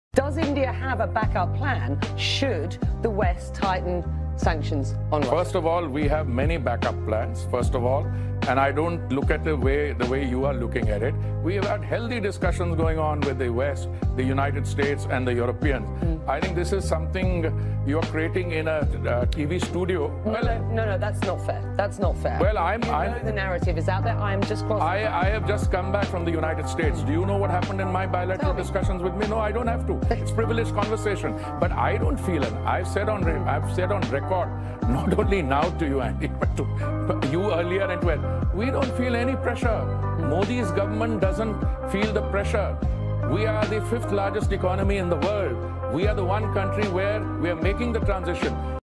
Hardeep Singh Puri on CNN India does not feel any pressure from west shorts
Salute to Hardeep Singh Puri at his candid best